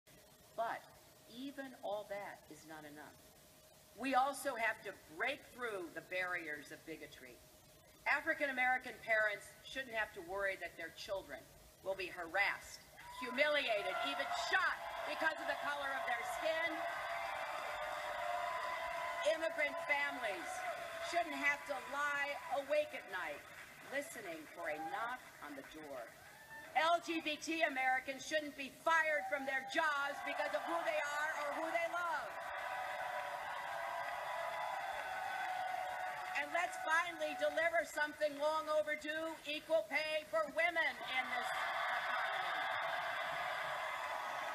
Hillary Clinton, speaking after getting benghazied in NH, goes into blacklivesmatter mode
NH, 2/9/16